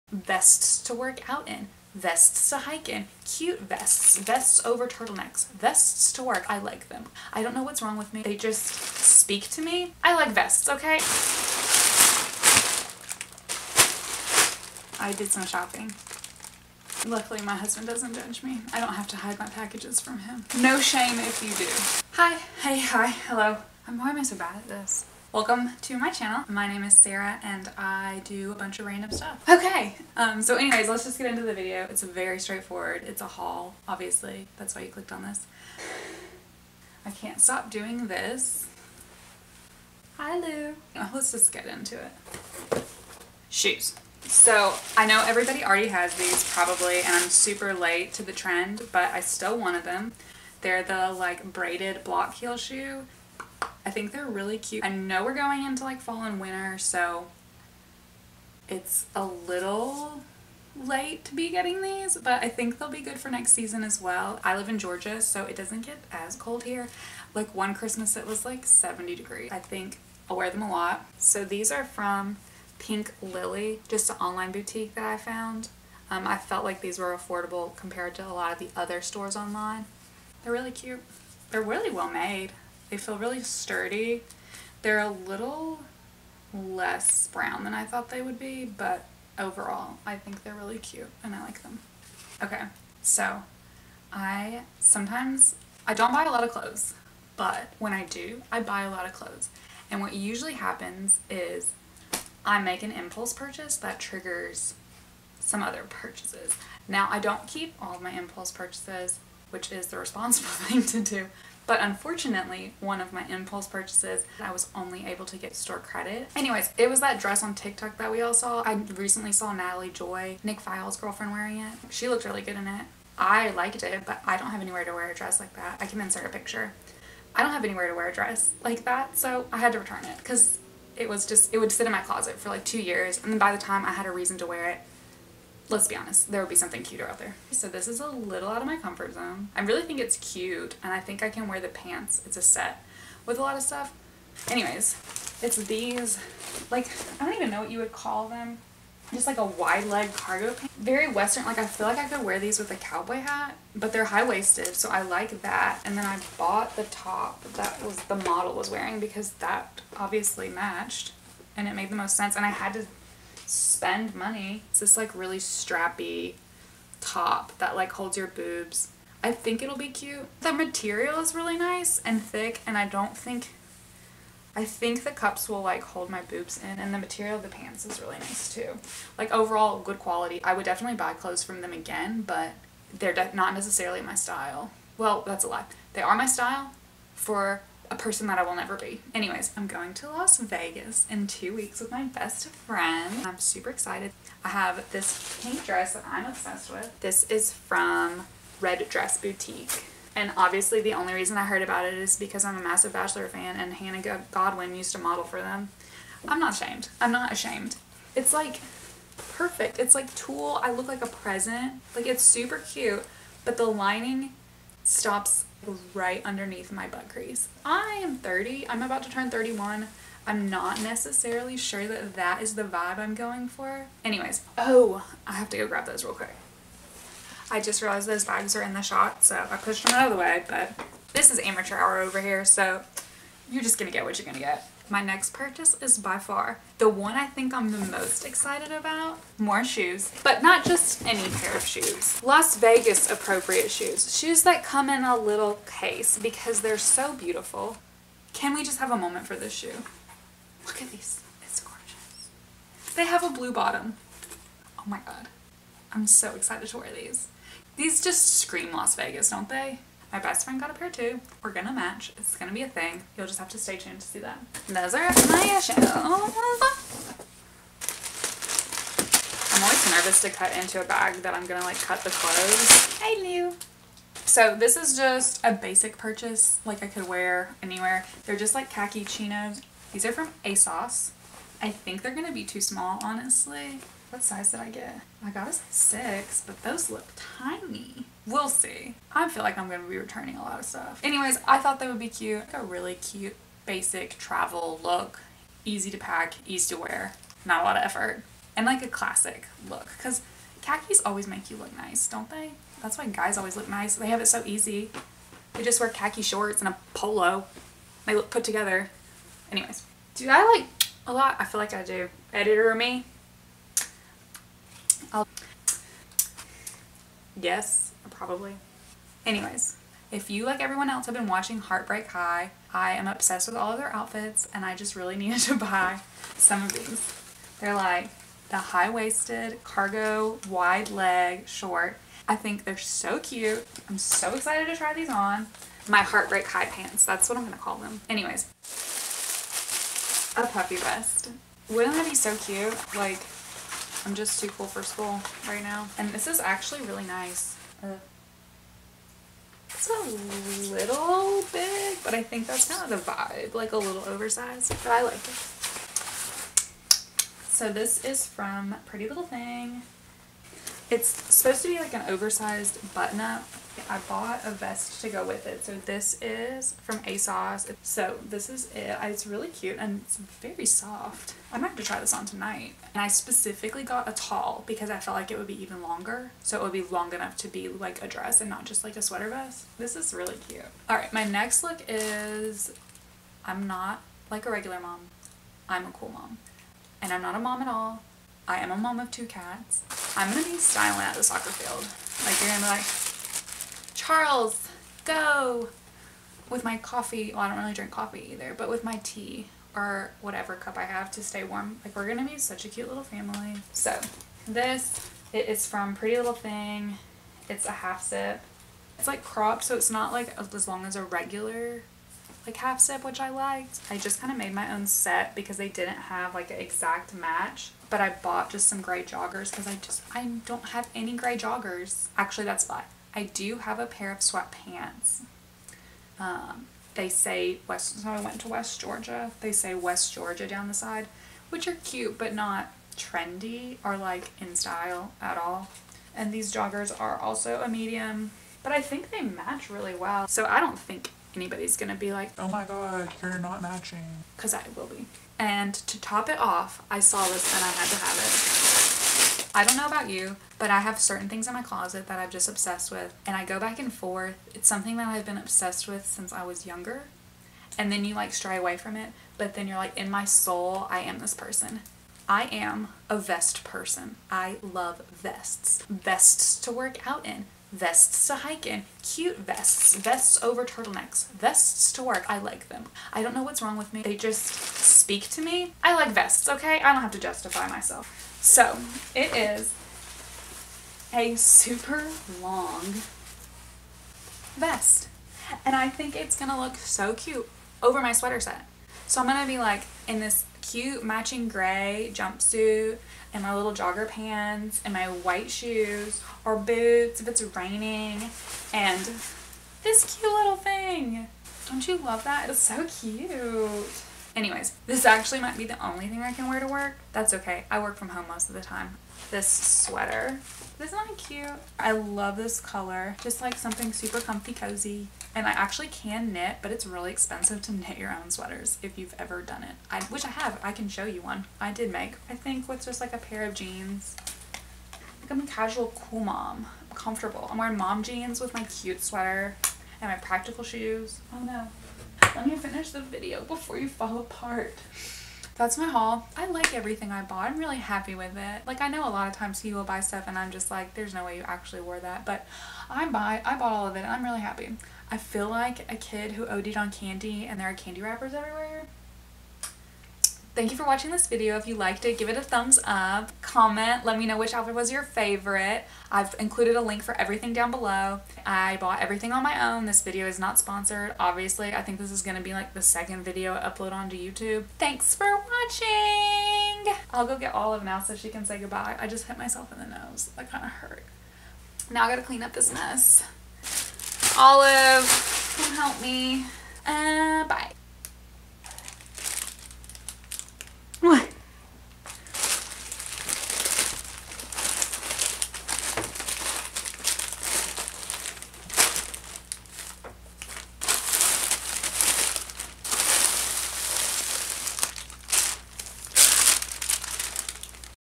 *Unboxing* Fall Haul - Everything I bought this month
Black Wide Leg Pants -Pretty Little Thing (Size 6)

Argyle Sweater Vest in Blue - Asos (Size 6 Tall)

This video might be a little all over the place, but I thought it might be fun  to show you all of the clothes I’ve purchased this month. We’re just here to have fun, so come along with me on this rollercoaster.

P.S. I did shoot a try on of the clothes, but…I didn’t like it. :)

xoxo,
Sarah, Zach, Lewis, & Olive